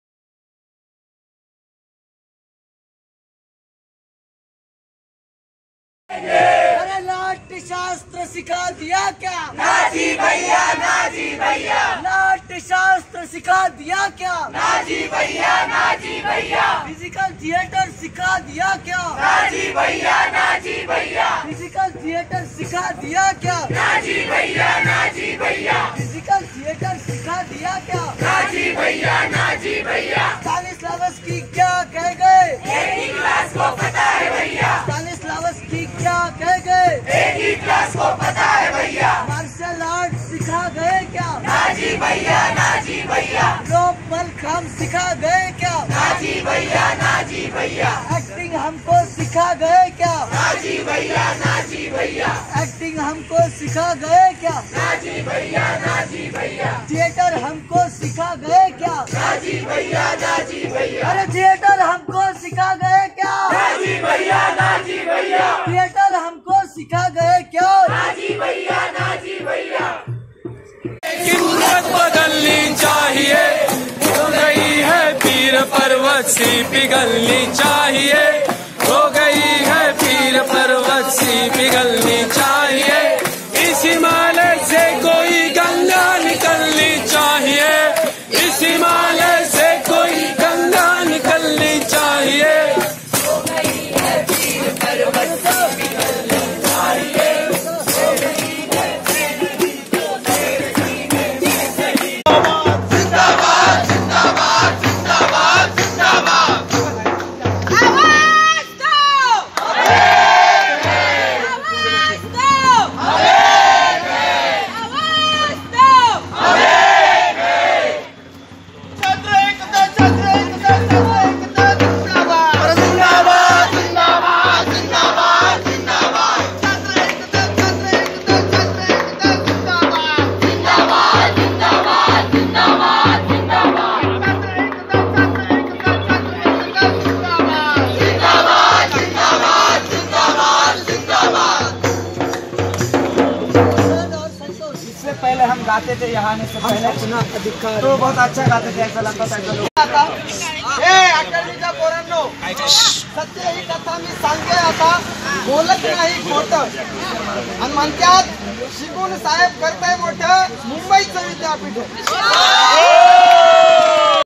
Protest by Mumbai University Students
Protest by Academy of Theater Art students

Mumbai University Academy of Theatre Art on Protest against the illegally Appointment of Director and Failure of Academical Studies.

Demands
Immediately Resignation of Director
We Need Visionary Leadership
Quality and Dignity of Education
Compensation of Loss

AISF Mumbai Extend Solidarity to Protest